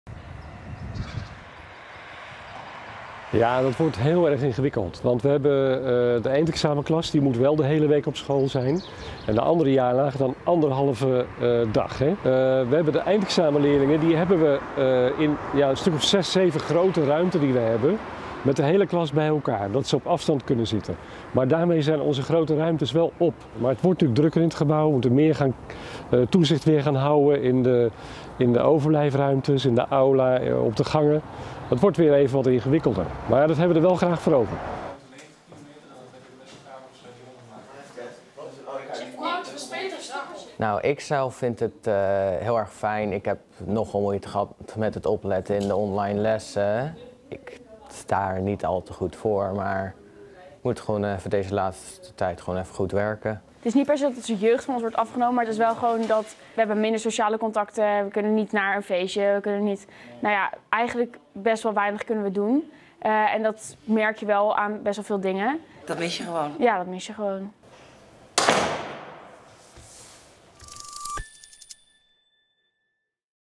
Als de middelbare scholen weer open gaan, wordt het een ingewikkelde puzzel
Nijmegen - "Anderhalve dag de middelbare school weer openen, het is weinig", dat vindt rector Ronald van Bruggen van het Stedelijk Gymnasium Nijmegen. "Het gaat zeker helpen, maar het is niet genoeg. Heel jammer dat niet meer kan met anderhalve meter afstand."